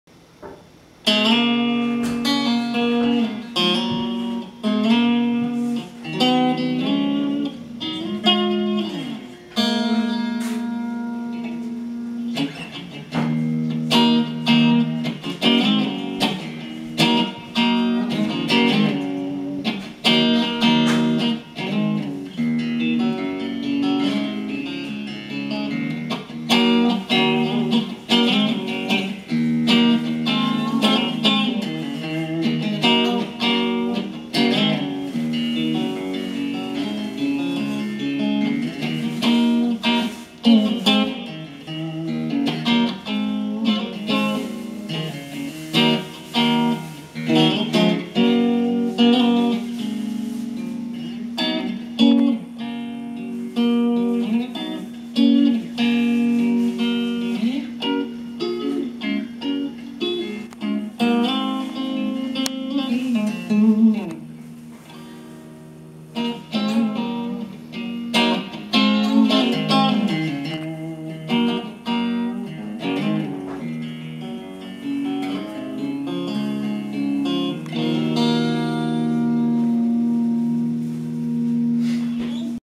James plays guitar for Julius
6.18.11 James Hingston playing a guitar he built. :-)